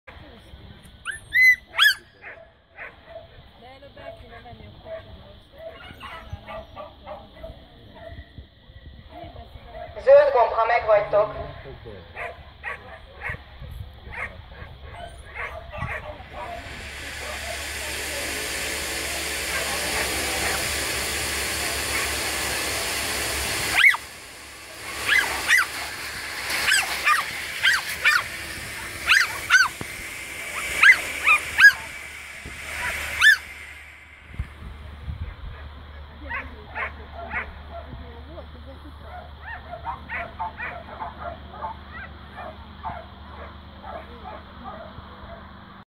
Staffordshire bull terrier racing
3 Staffordshire bull terrier racing in Alsónémedi (Hungary). They break out of starting boxes, wearing muzzles and numbered blankets. They run 80 m.